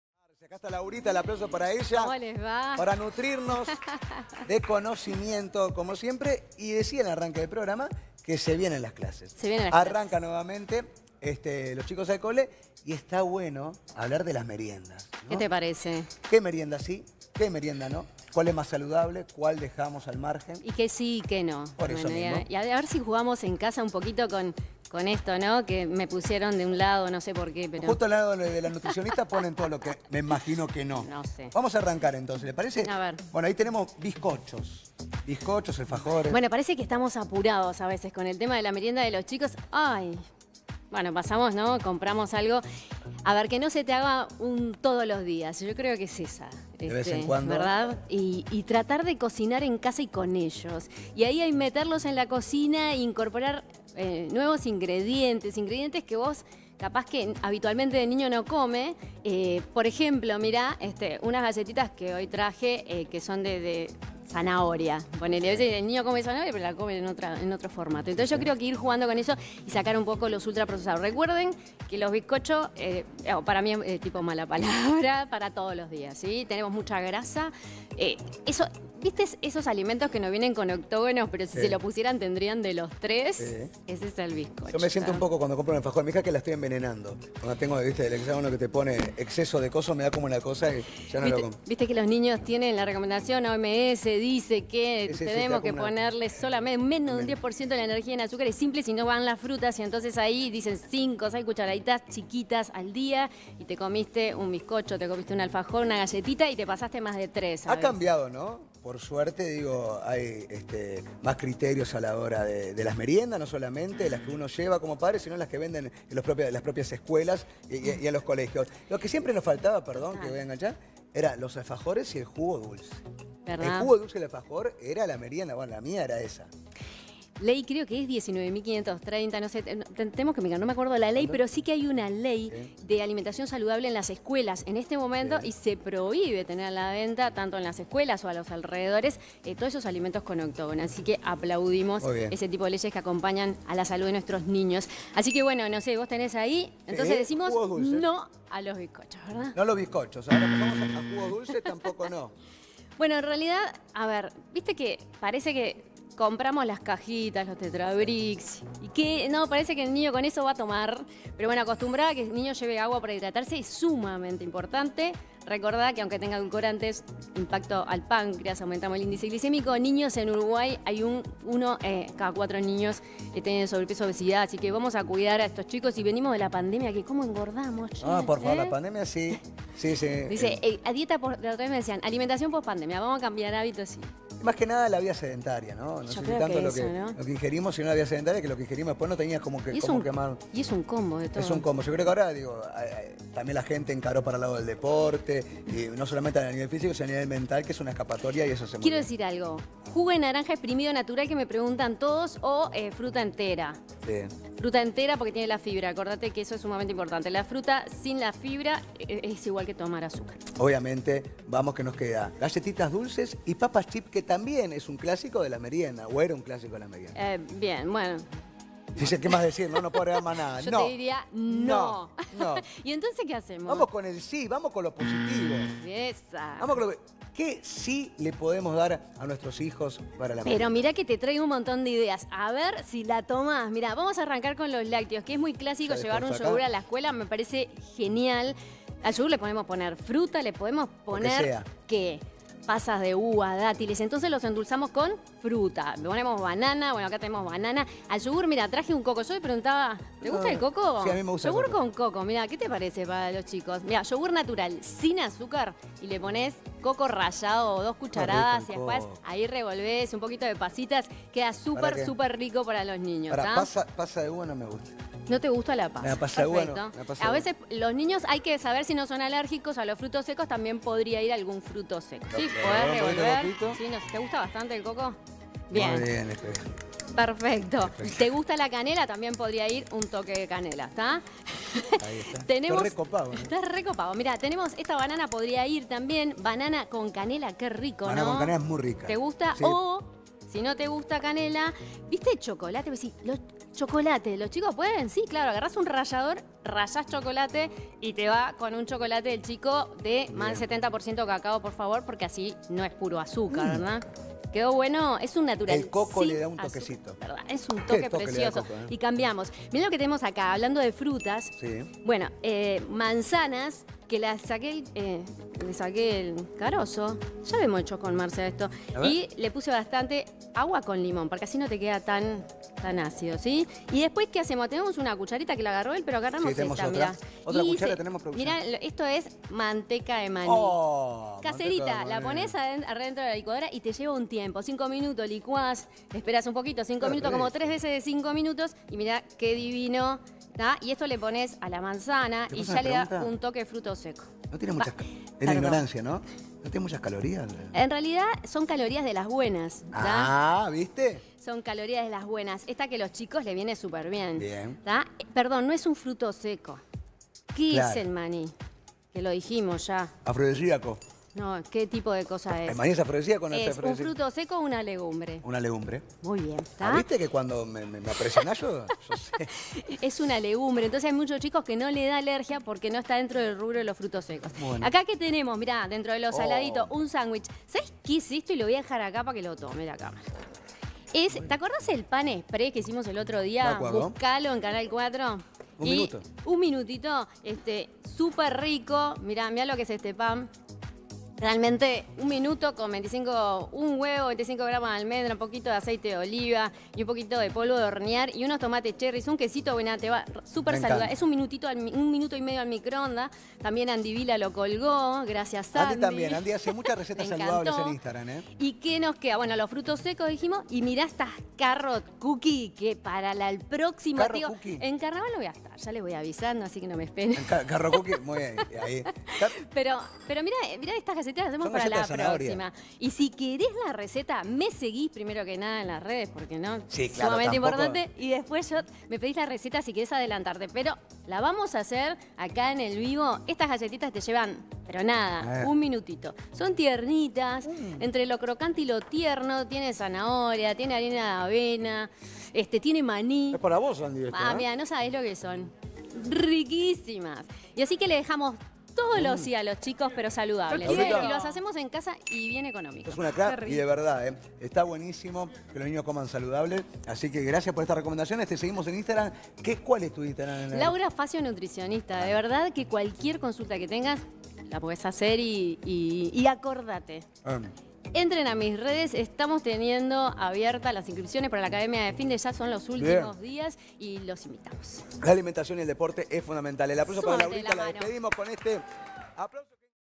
Vamo Arriba - Los sí y los no de la merienda
Recomendaciones de nuestra Nutricionista Laura Fazio.